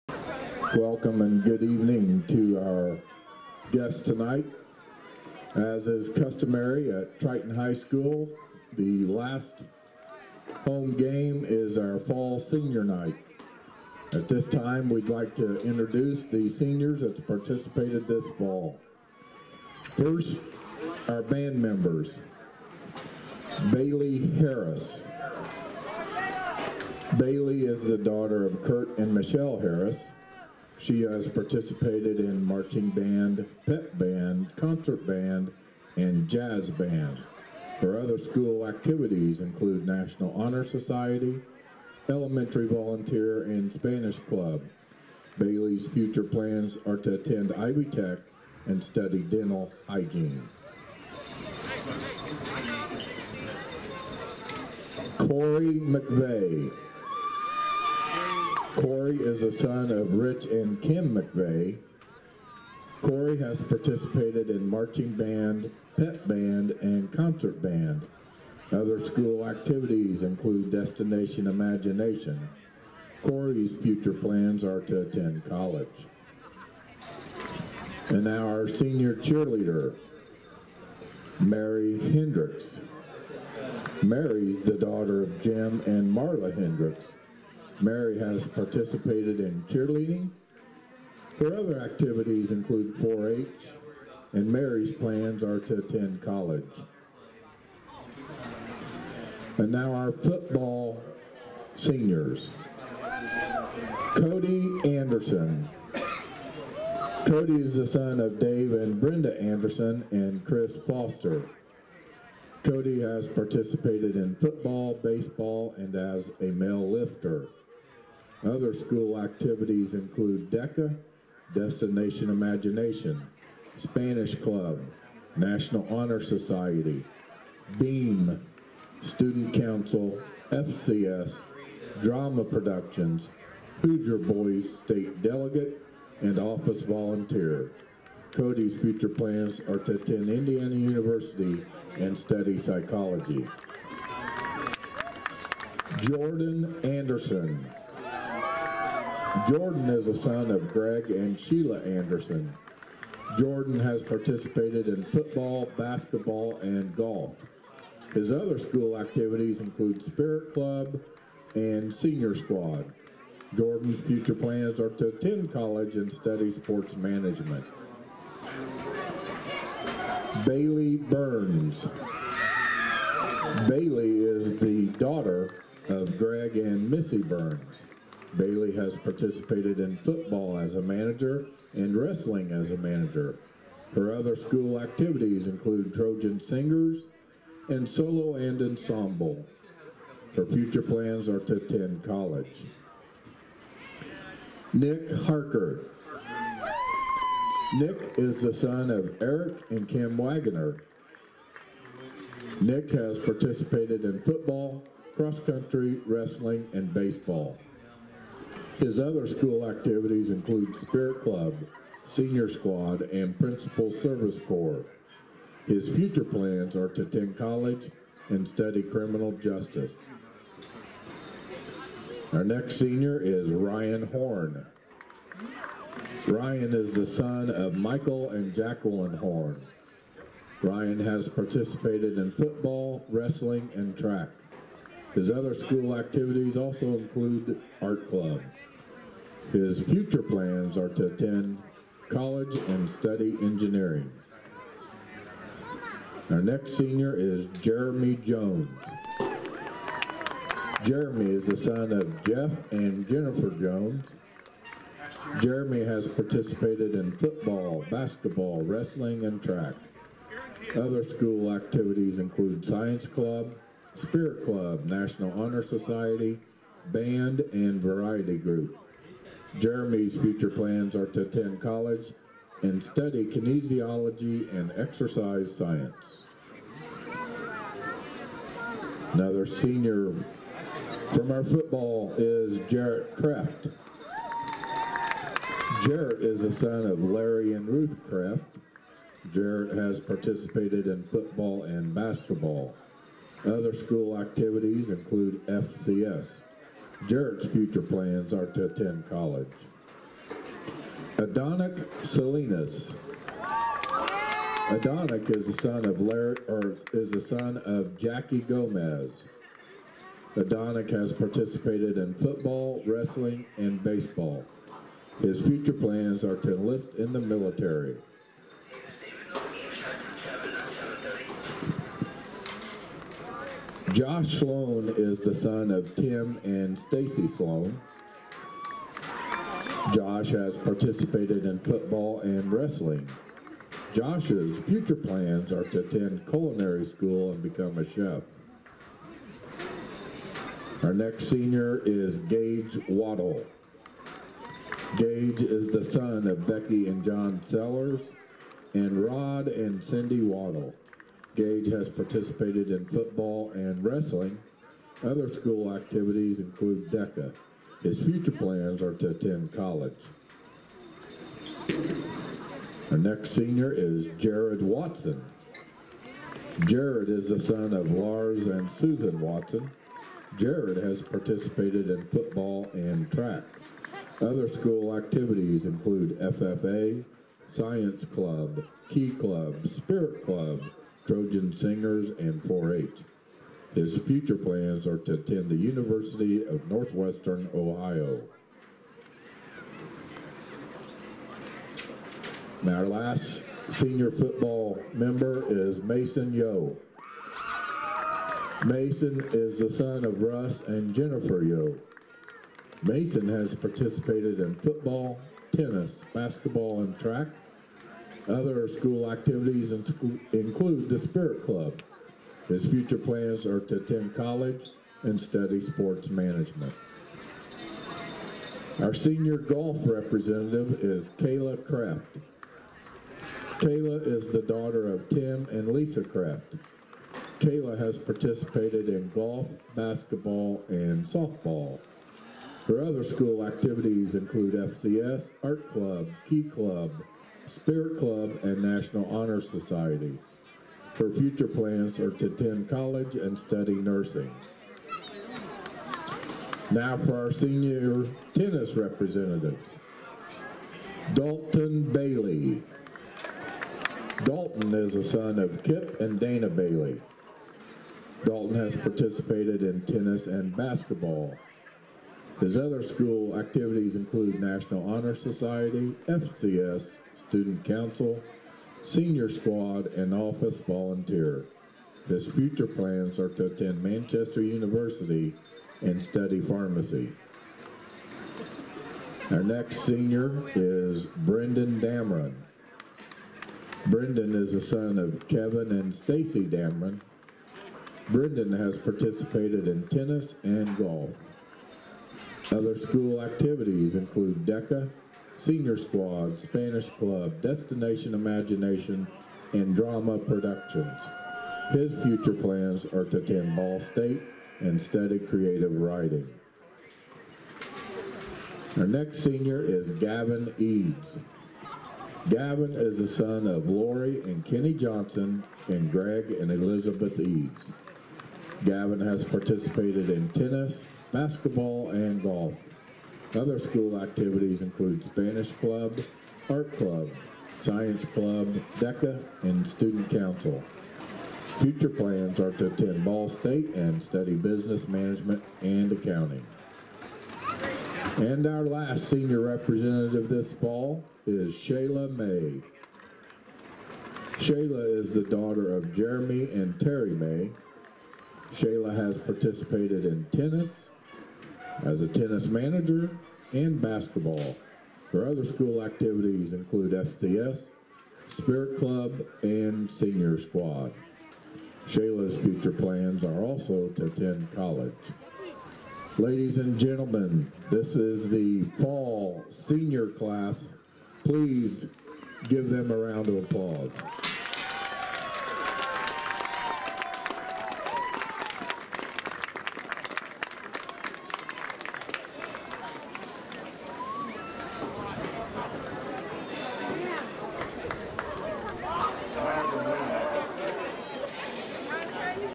Triton Fall Senior Recognition 10/16/2015
Band: Baylee Harris, Corey McVey; Cheer: Mary Hendrix; Football: Cody Anderson, Jordan Anderson, Bailee Burns, Nick Harker, Ryan Horne, Jeremy Jones, Jarrett Kreft, Adonic Salinas, Josh Slone, Gage Waddle, Jarred Watson, Masen Yeo; Girls Golf: Kayla Kreft; Boys Tennis: Dalton Bailey, Brenden Damron, Gavin Eads, Shayla May